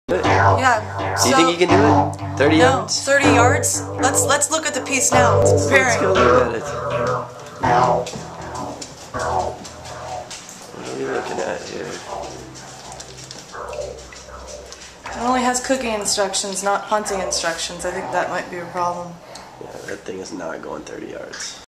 $4 Bet- Part One- The Pre-game
Charlie bets Matt $4 that he can't punt a baby fetus (cornish game hen) 30 yards... can he do it?